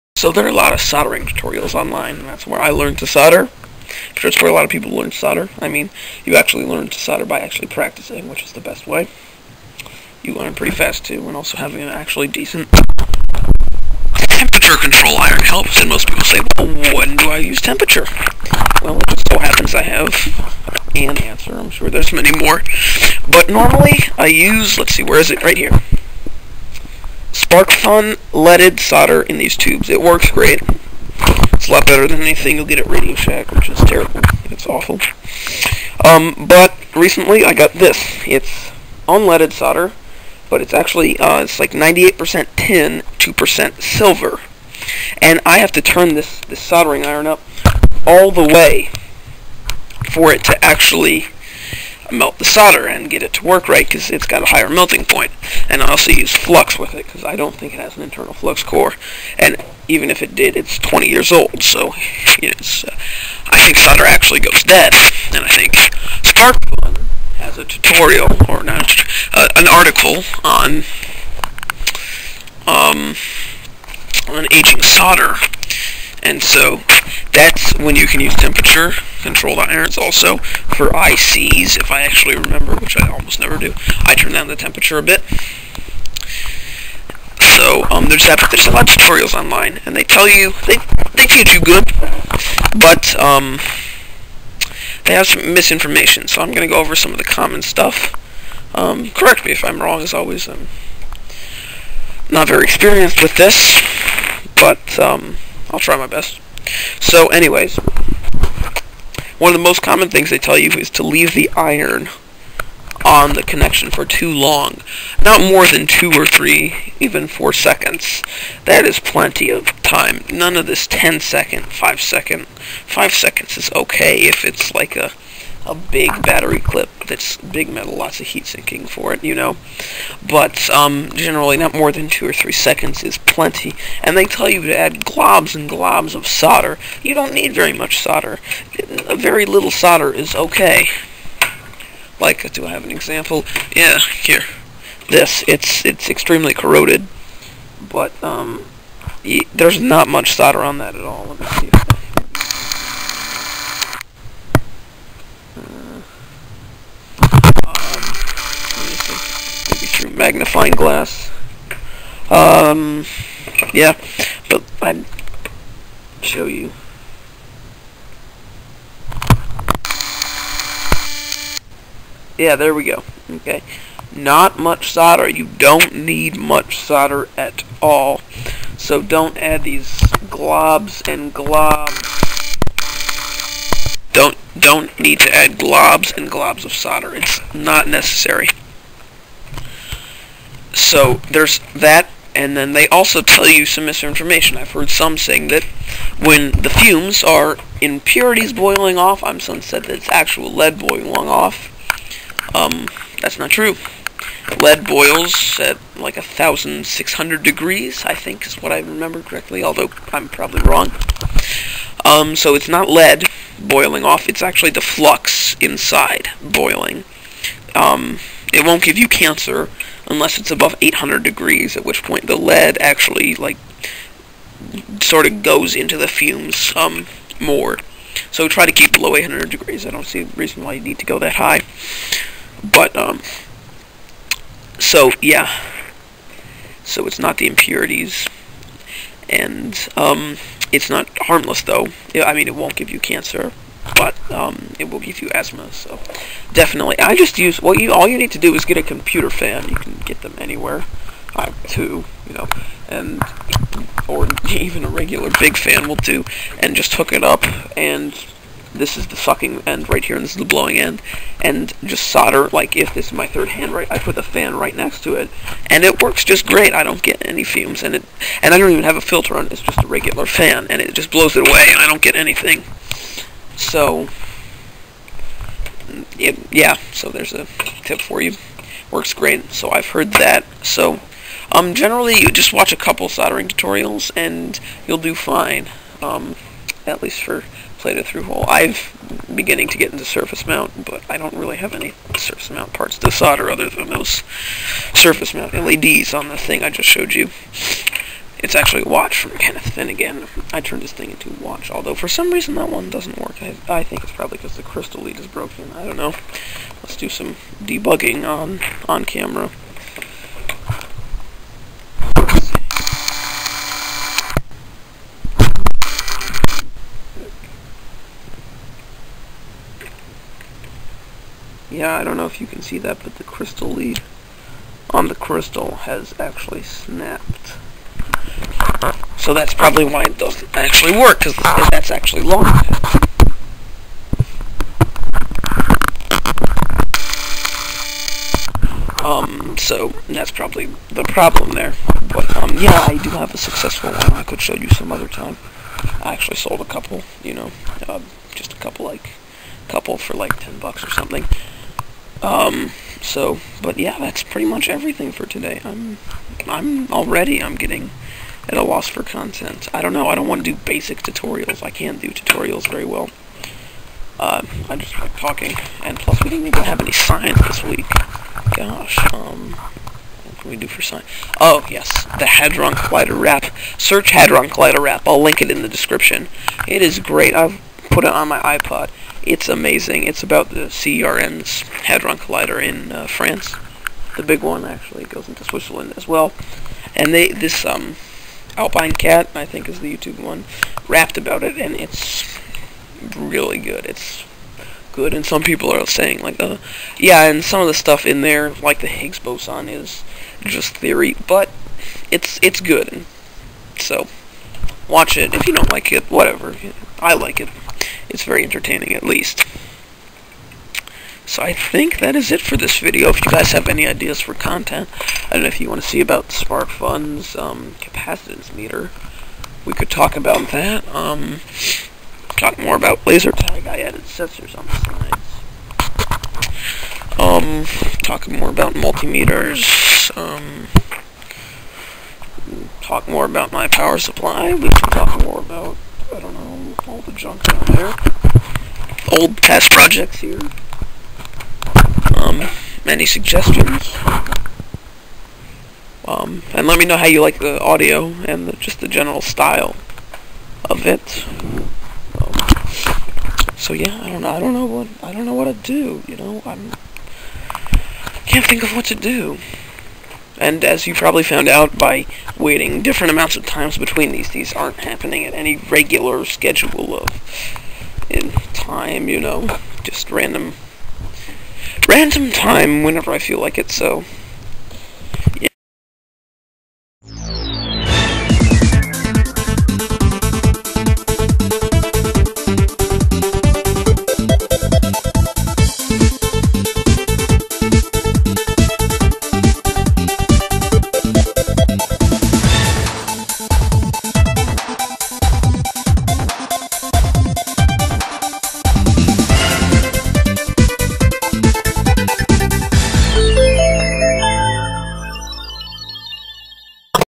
Tutorial On Soldering Tutorials
There are plenty of great soldering tutorials online but I expose some common mistakes. Remember not to keep the iron on the joint for too long and not to add too much solder.